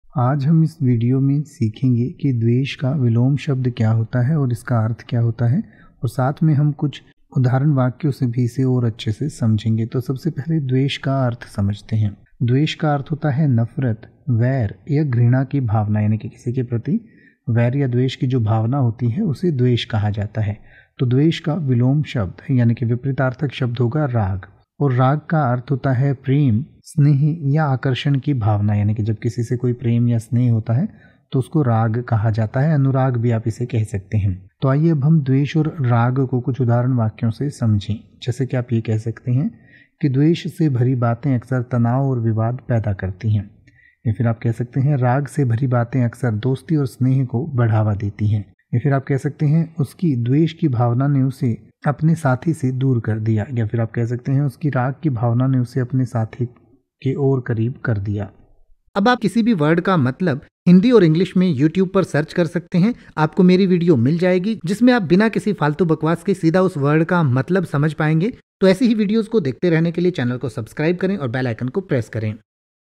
द्वेष का विलोम शब्द क्या होता है | द्वेष का अर्थ | द्वेष का अर्थ और विलोम शब्द समझें
आज हम इस विडियो में सीखेंगे कि द्वेष का विलोम शब्द क्या होता है और इसका अर्थ क्या होता है और साथ में हम कुछ उदाहरण वाक्यों से भी इसे और अच्छे से समझेंगे।

द्वेष का अर्थ होता है नफरत, वैर, घृणा की भावना

तो द्वेष का विलोम शब्द यानि कि विपरीतार्थक शब्द होगा राग और राग का अर्थ होता है प्रेम, स्नेह, या आकर्षण की भावना

आइये अब हम द्वेष और राग को कुछ

उदाहरण वाक्यों से समझें

जैसे कि आप कह सकते हैं :

द्वेष से भरी बातें अक्सर तनाव और विवाद पैदा करती हैं।

राग से भरी बातें अक्सर दोस्ती और स्नेह को बढ़ावा देती हैं।

उसकी द्वेष की भावना ने उसे अपने साथी से दूर कर दिया।

उसकी राग की भावना ने उसे अपने साथी के और करीब कर दिया।